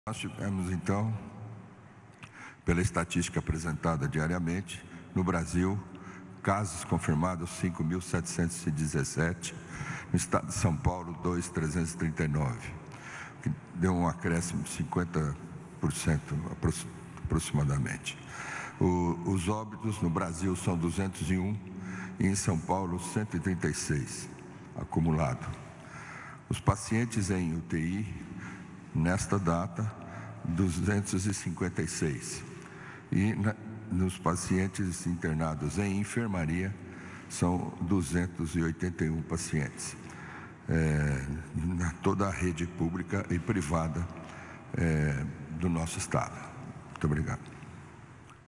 136 mortes e 2.339 casos confirmados em São Paulo por Covid-19
São Paulo segue como o estado mais afetado, até o momento, pela pandemia do novo coronavírus. Ao menos 136 pessoas já morreram em consequência da doença, segundo o governo paulista.